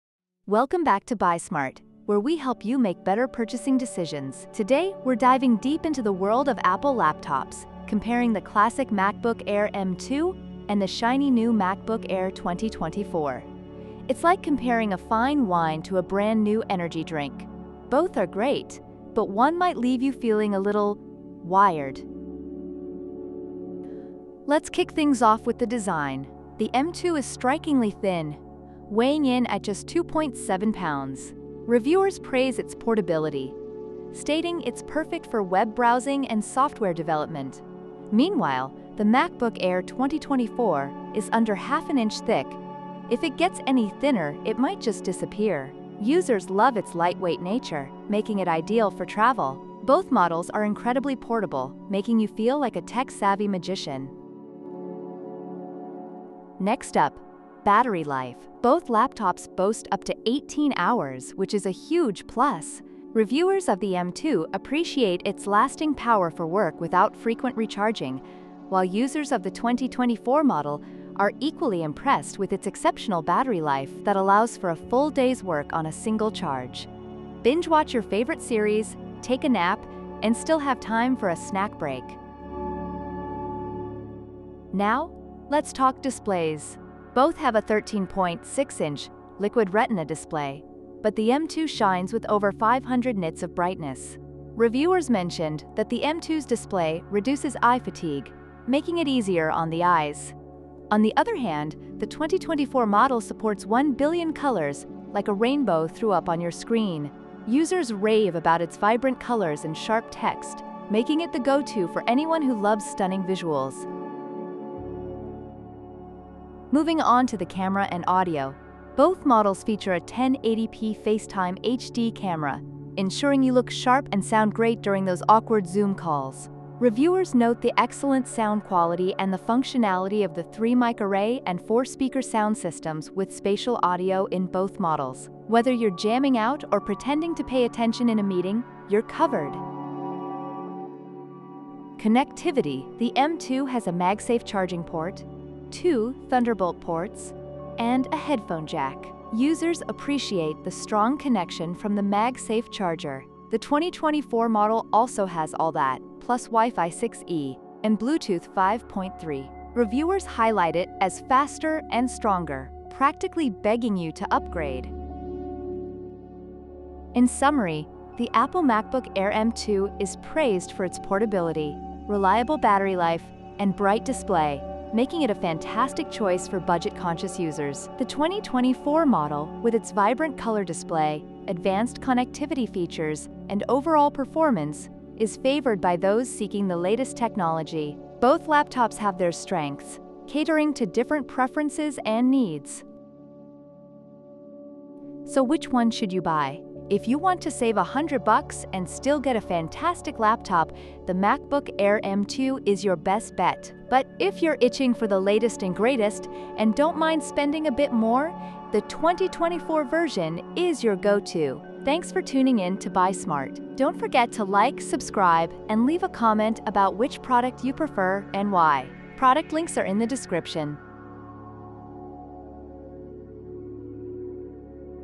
Apple M2 vs M3 MacBook Air: Which One Wins? 🖥️✨
In this video, we compare the 2022 MacBook Air with M2 chip and the 2024 MacBook Air with M3 chip! Discover which model offers better performance, design, and features for your needs! 💻

Join us as we dive into specifications, battery life, display quality, and overall value to help you make an informed decision. 🔍📊